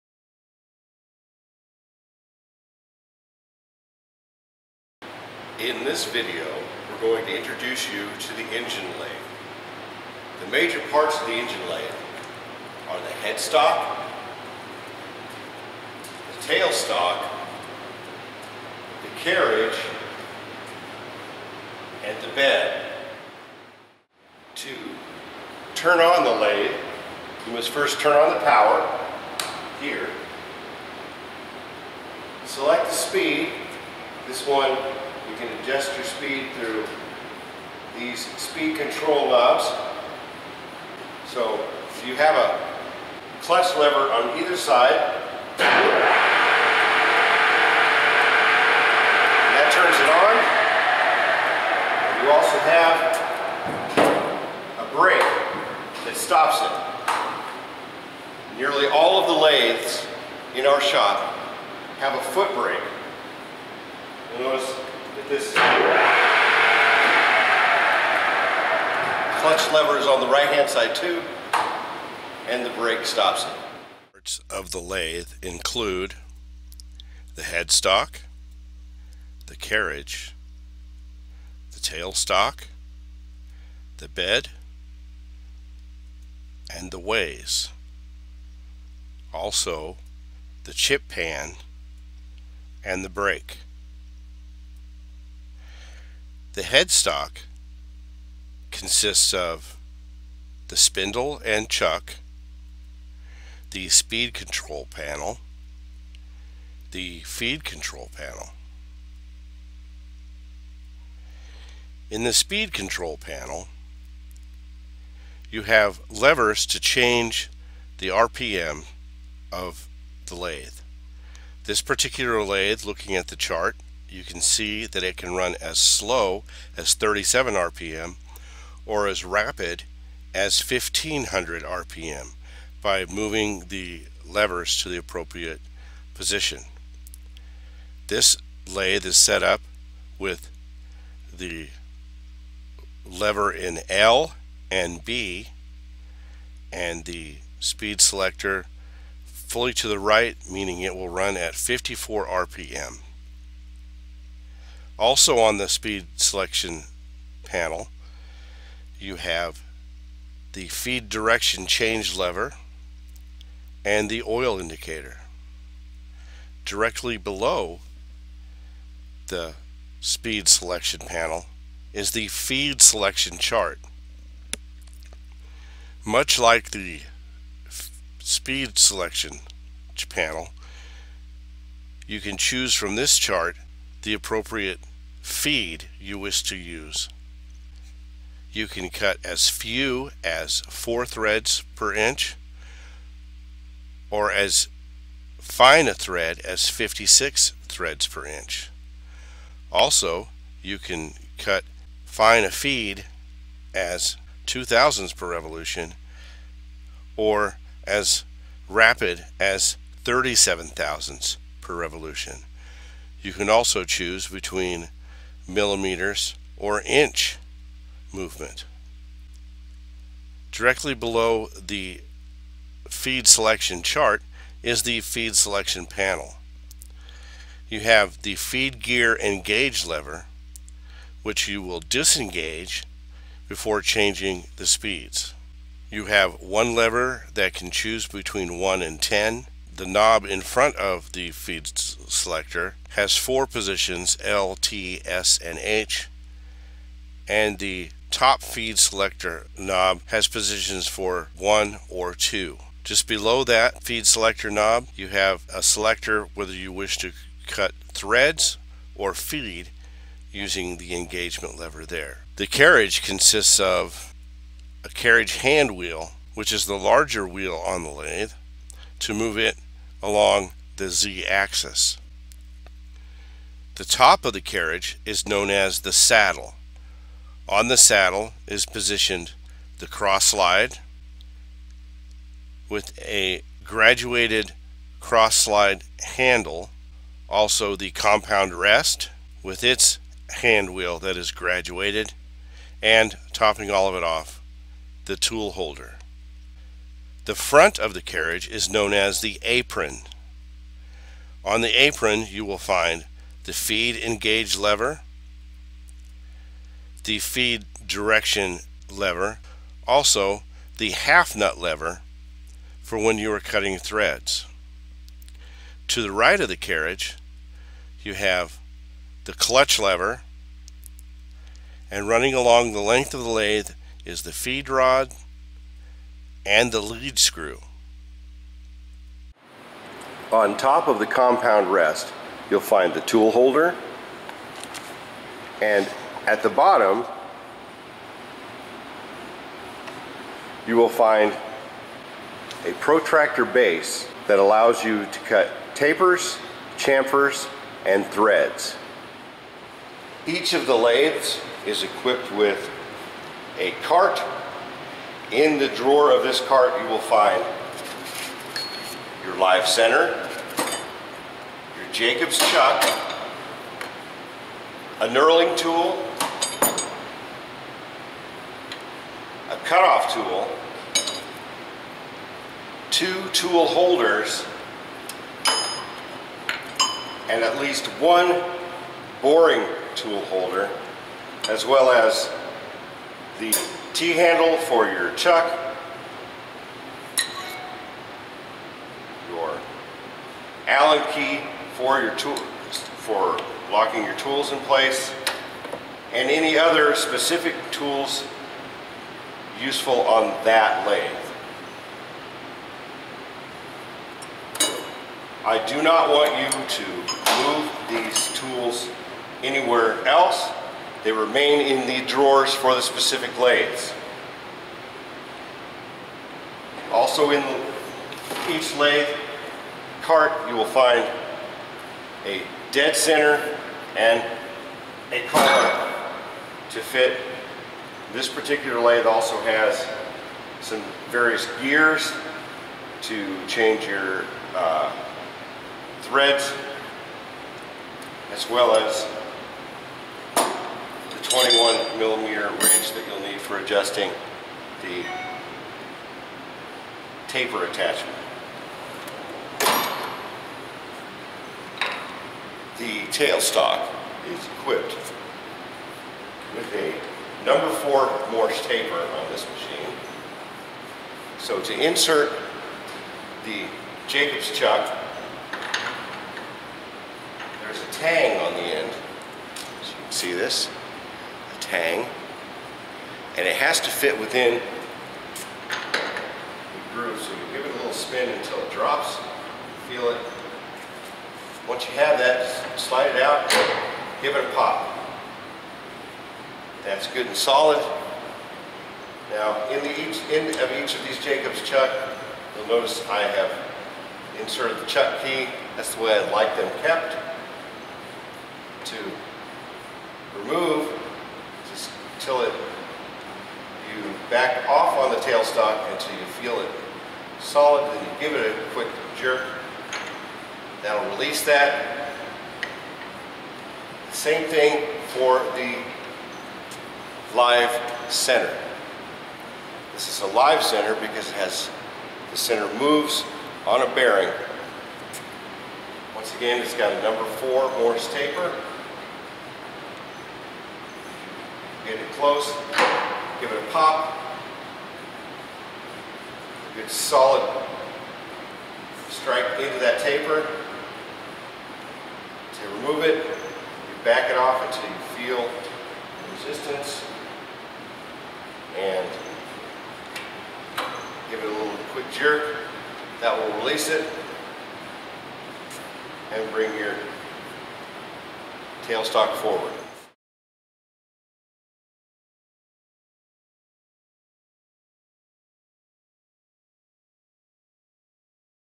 Introduction to the Lathe
An introduction to the engine lathe and its controls